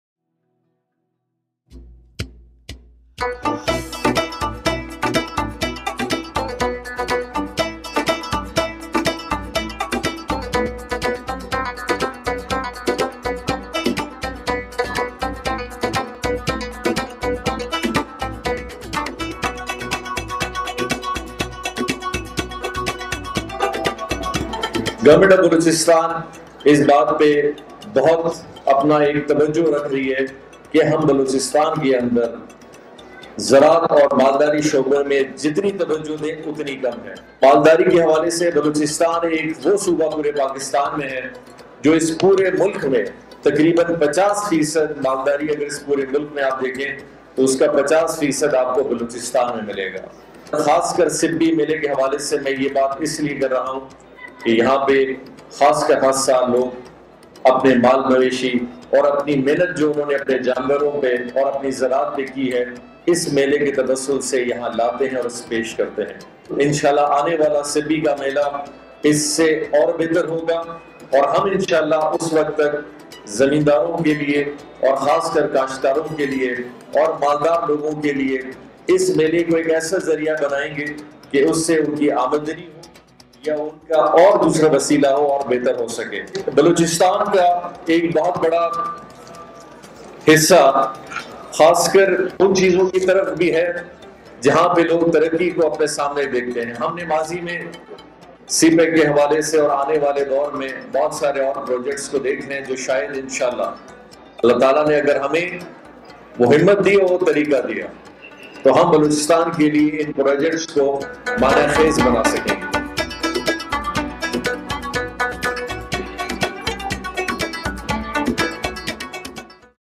سبی : سی پیک کو ہم معانے خیز بنا کر ہی چھوڑیں گے۔ وزیراعلی بلوچستان کا سبی میلے کی تقریب سے خطاب
CM Balochistan inaugurates Industrial Exhibition in SIBI Mela 2019
بلوچستان کے لوگ ترقی کو اپنے سامنے دیکھ رہے ہیں

We'll brought to you most beautiful, informatics, funny, musical and interesting videos.